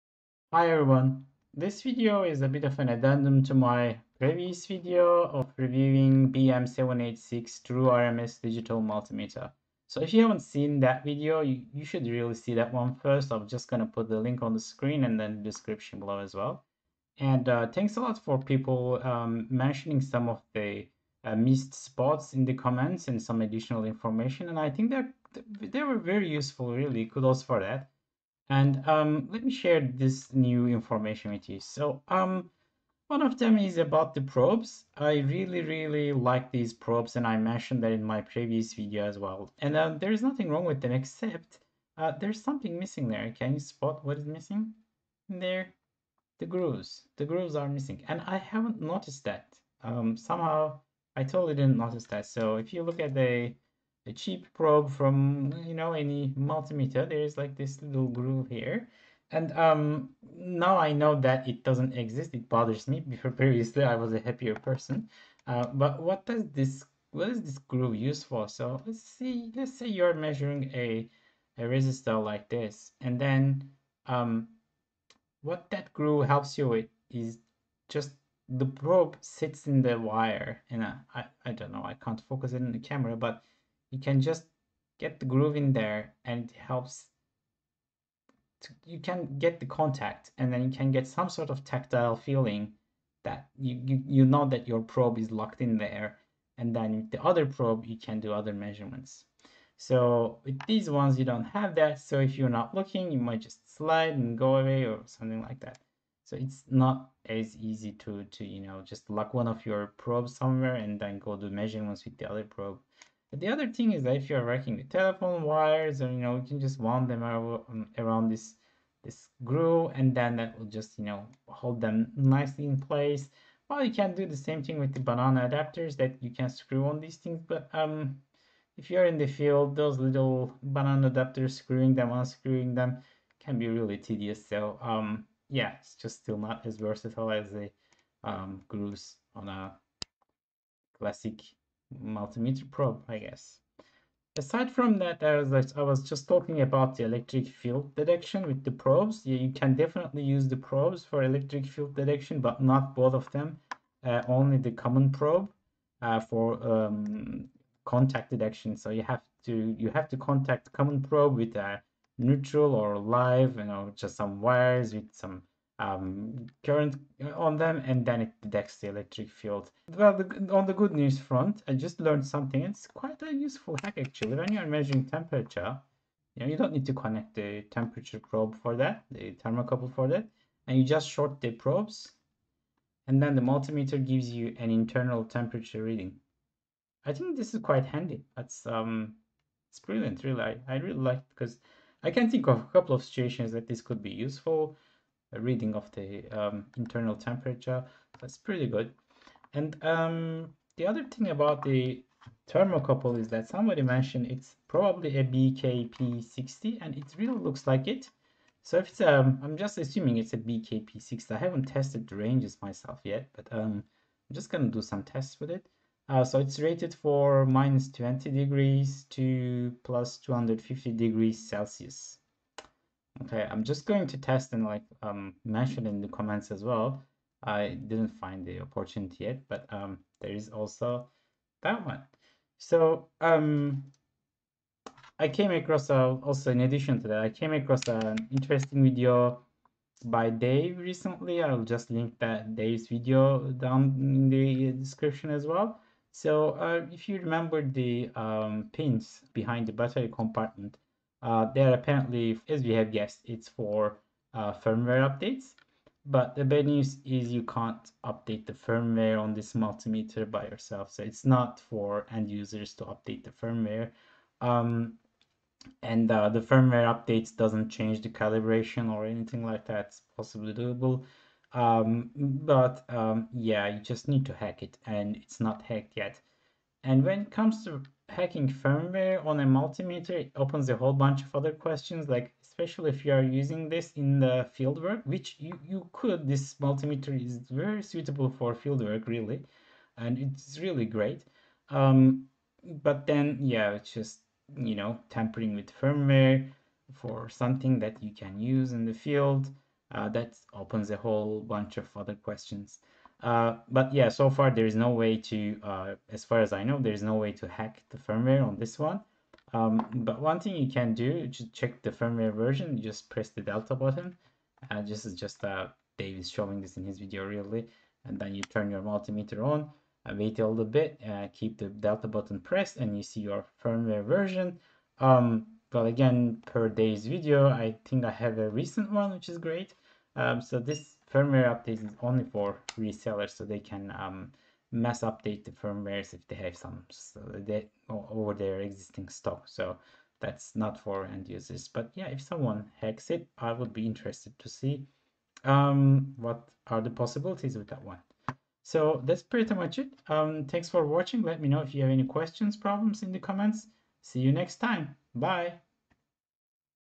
Brymen BM786 True RMS Digital Multimeter Review - Part 2
Addendum to my previous general review of Byrmen BM786 true RMS digital multimeter I will be talking a bit more about probes and a few tricks.